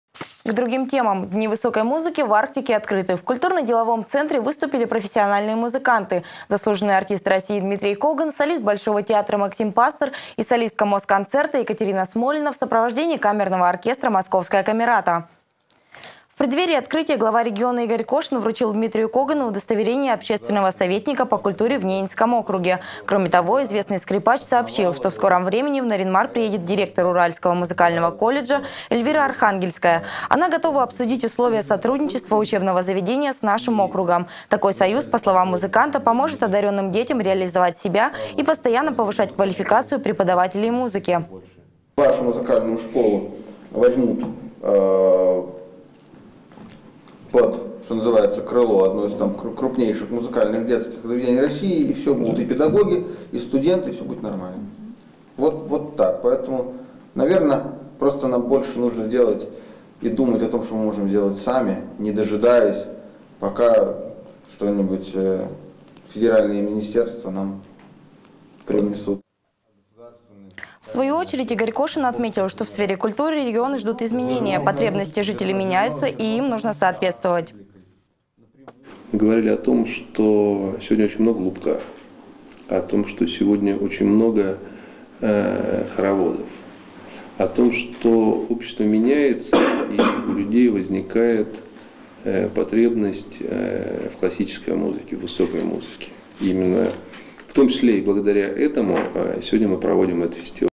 Дмитрй Коган стал общественным советником по культуре в НАО
Дни высокой музыки в Арктике открыты. В культурно-деловом центре выступили заслуженный артист России Дмитрий Коган, солист Большого театра Максим Пастер и солистка Москонцерта Екатерина Смолина в сопровождении камерного оркестра «Московская камерата».